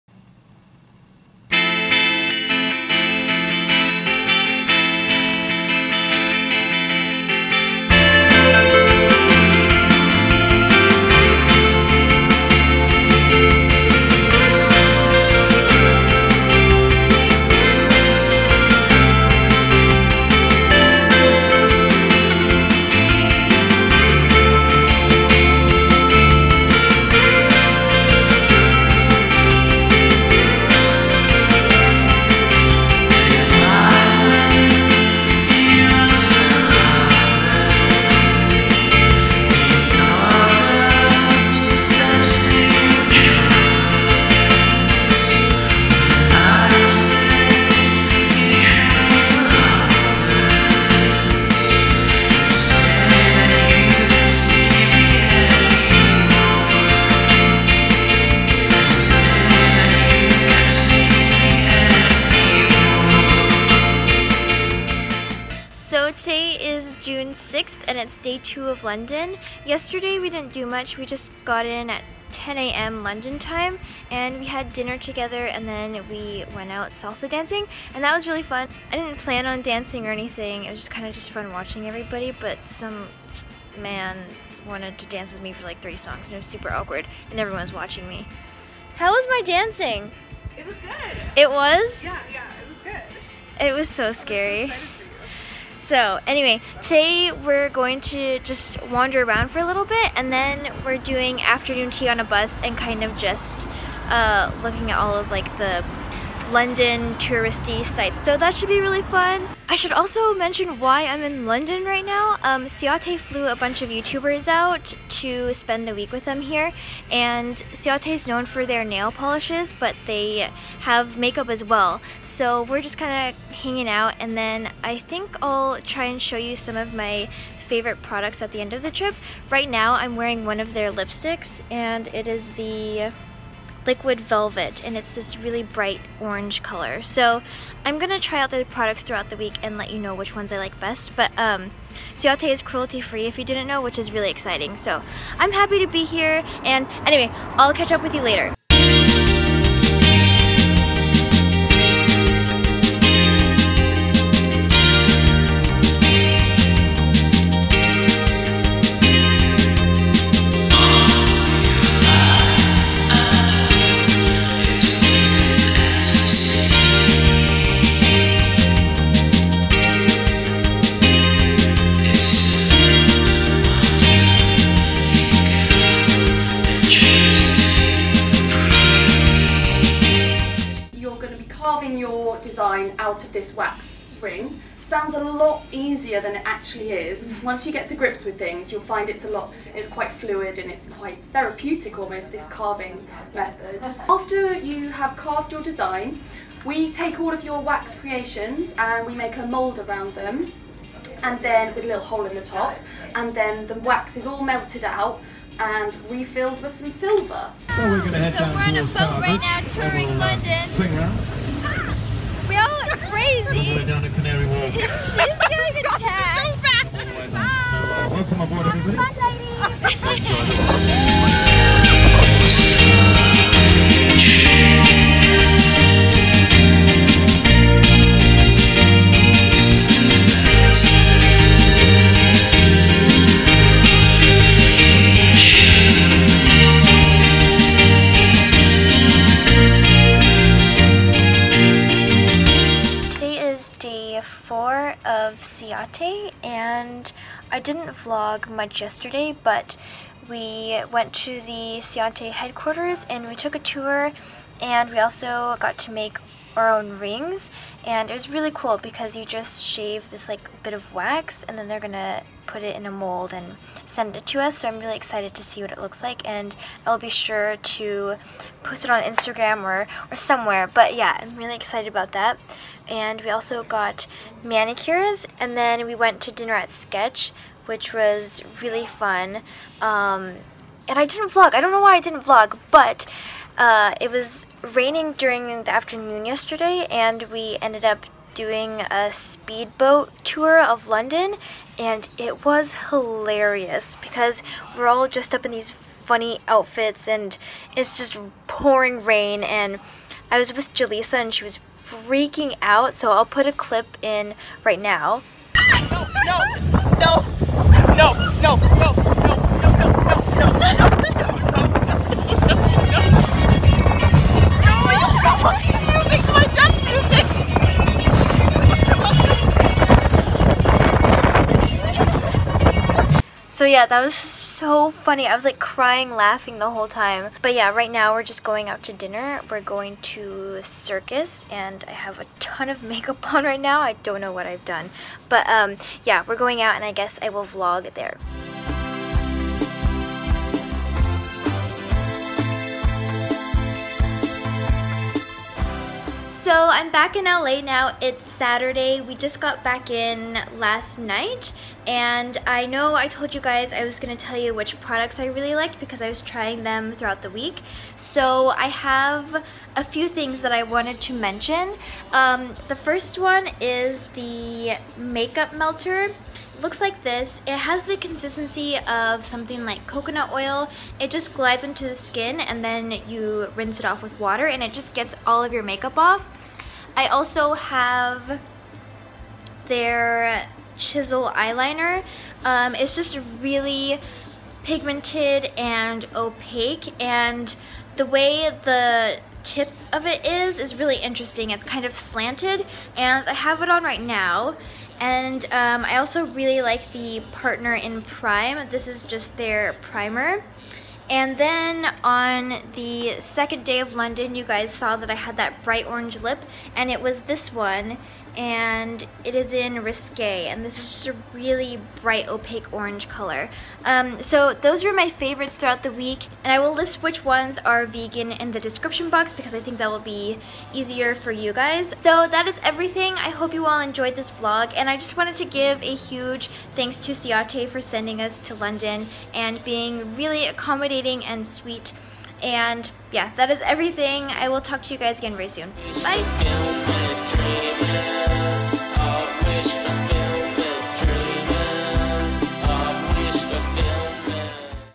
🎀 VLOG || Touring London with Ciaté
Hello, friends!
Exploring Lodon with Ciaté - Woohoo!

Makeup Melter
Chisel Eyeliner
(V) Partner In Prime
(V) Liquid Velvet Matte Liquid Lipstick

☝ SHOW THEM LOVE